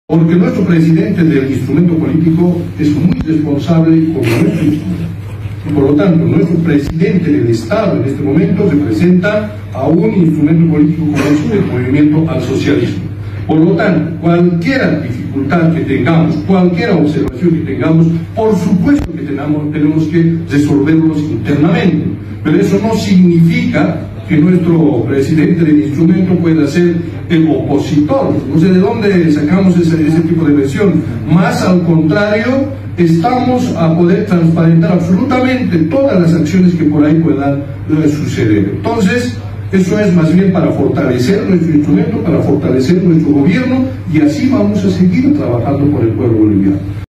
𝗡𝗶𝗲𝗴𝗮 𝗾𝘂𝗲 𝗱𝗲𝗻𝘂𝗻𝗰𝗶𝗮 𝗱𝗲 𝗰𝗼𝗿𝗿𝘂𝗽𝗰𝗶𝗼́𝗻 𝗲𝗻 𝗔𝗕𝗖 𝗱𝗲 𝗘𝘃𝗼, 𝗴𝗲𝗻𝗲𝗿𝗲𝗻 𝗱𝗶𝘃𝗶𝘀𝗶𝗼́𝗻 𝗲𝗻 𝗲𝗹 𝗠𝗔𝗦-𝗜𝗣𝗦𝗣.
Freddy Mamani, masista Presidente de la Cámara de Diputados de Bolivia,  niega que las denuncias realizadas por el ex dictador tiránico Evo Morales, sobre presuntos graves hechos de corrupción en la Administradora Boliviana de Carreteras (ABC), que involucran a Edgar Montaño, Ministro de Obras Públicas y funcionarios de alto nivel de la ABC, todos ellos designados por el régimen Arce Catacora, hayan generado división entre los partidarios del Movimiento al Socialismo (MAS-IPSP), sugiriendo que Morales no estaría tratando de gestar ningun tipo de situación contra la gestión de Catacora.

Nota de opinión ciudadana:

Con esta declaración, parece que los masistas pretenden demostrar que no existe ningún tipo de fricción interna en el partido azul, entre Evo Morales y su gente Vs. Luis Arce Catacora, sus seguidores y los de Choquehuanca, algo que obviamente cada vez es más evidente, qué va escalando sistemáticamente y que va menoscabando la oscura y perniciosa unidad de los masistas.